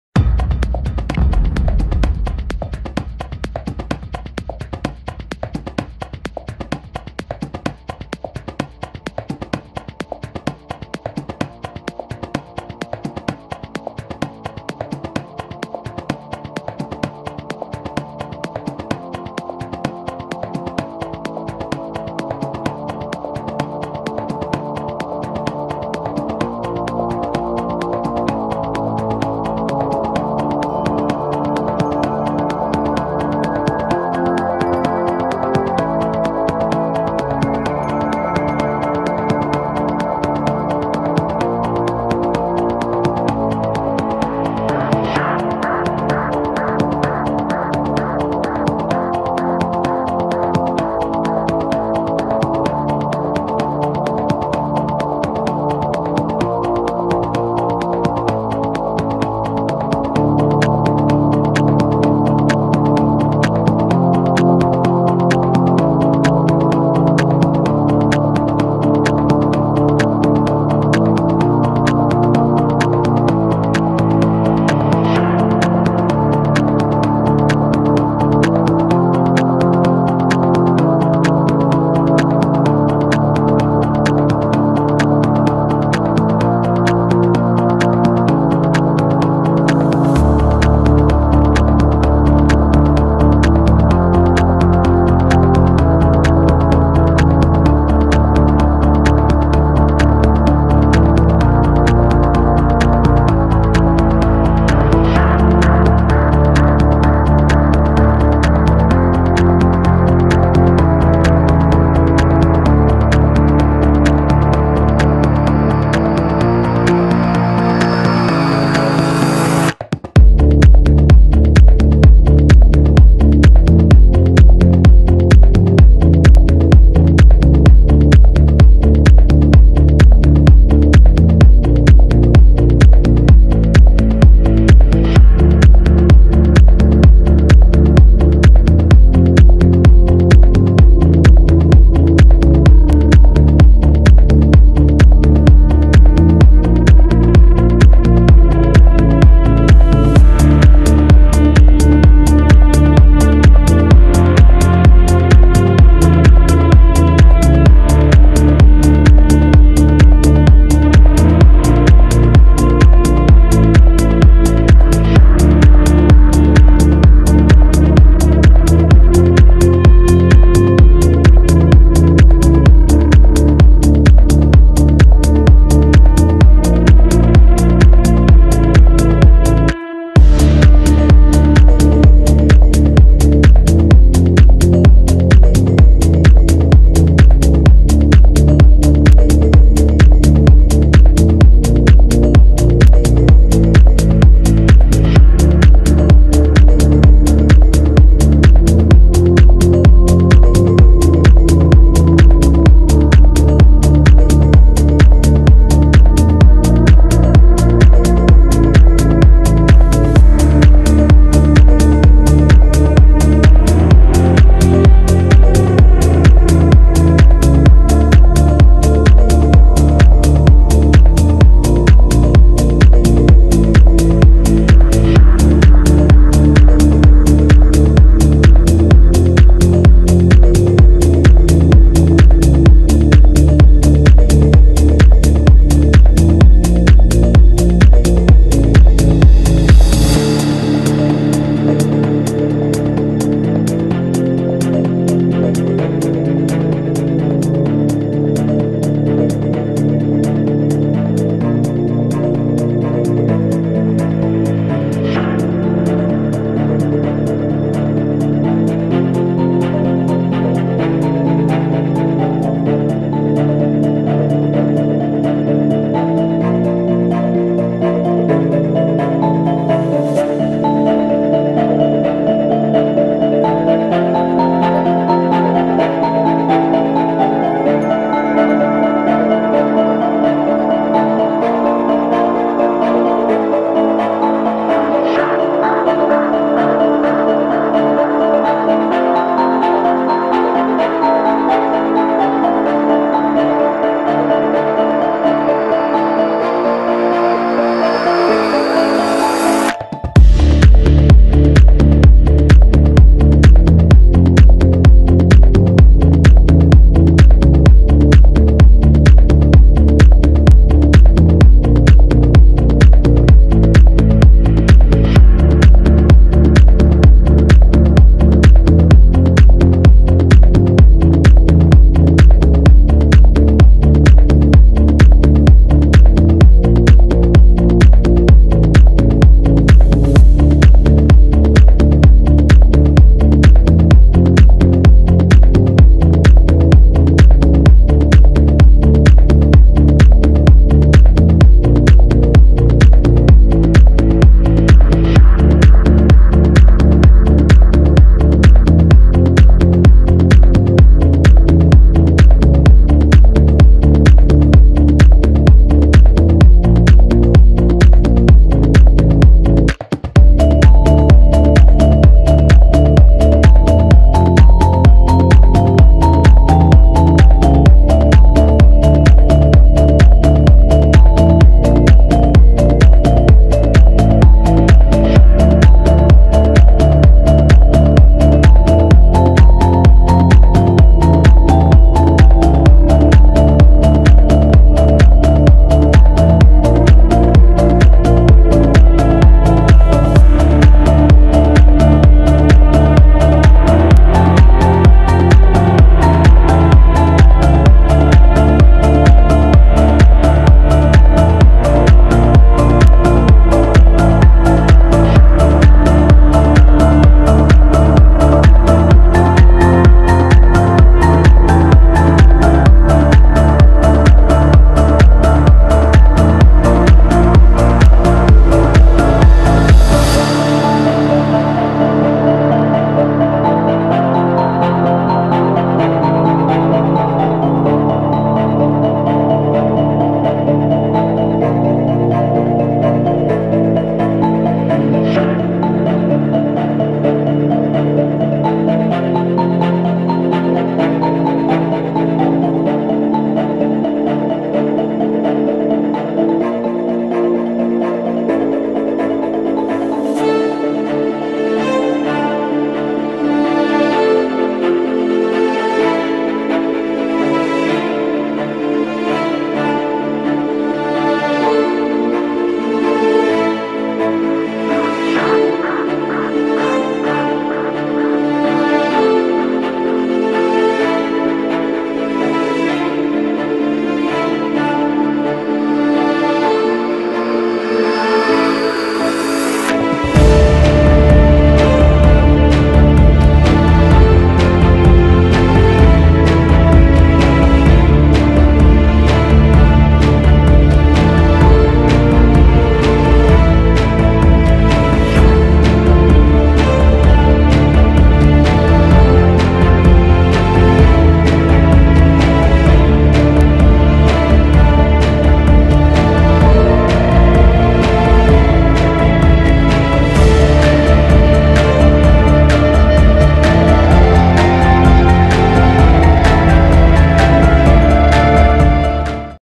1,000 Foot Radio Tower Climb!
The real time ride up a 1,200 foot tower to the antenna platform with a quick walk around.

Filmed 100% on a GoPro HERO 3
Color Corrected with DaVinci Resolve